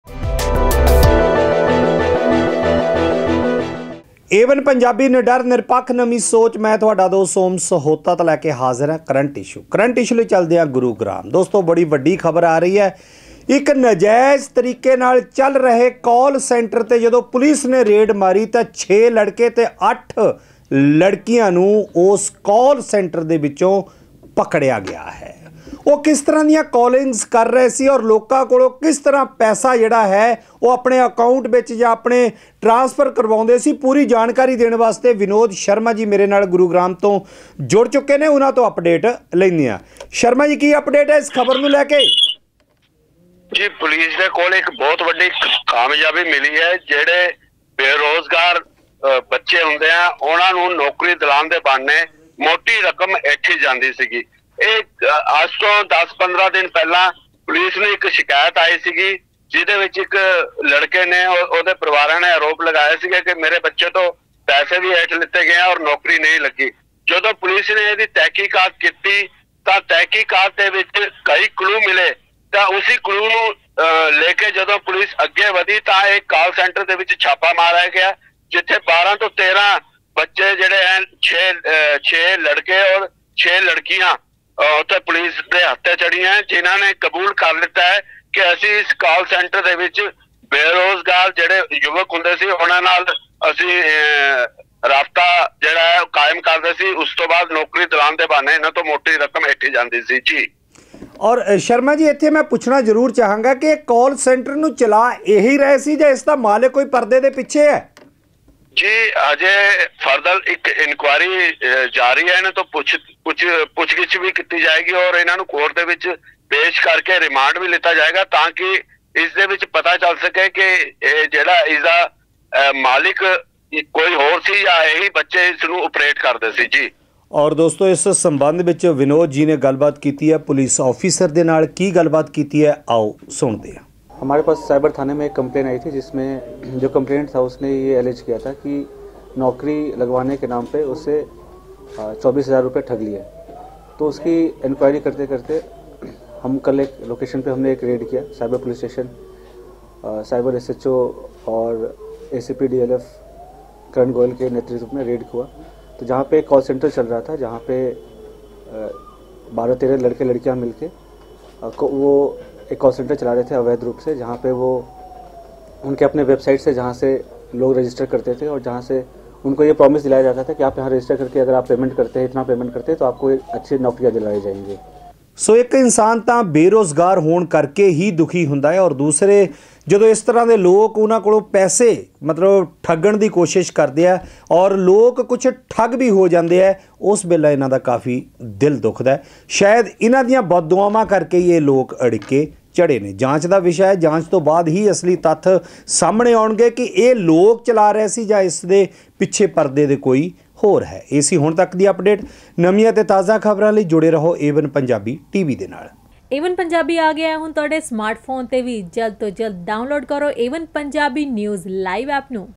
Gurugram | 6 ਲੜਕੇ ਤੇ 8 ਲੜਕੀਆਂ ਨੂੰ ਪੁਲਿਸ ਨੇ ਕੀ ਕਰਦੇ ਦਬੋਚਿਆ | AOne Punjabi Tv
ਫੋਨ ਕਾਲ ਕਰਕੇ ਚਲਾ ਰਹੇ ਸੀ ਨਜਾਇਜ਼ ਧੰਦਾ
ਪੁਲਿਸ ਦੇ ਸੀਨੀਅਰ ਅਫਸਰਾਂ ਨੇ ਕੀਤੀ ਰੇਡ |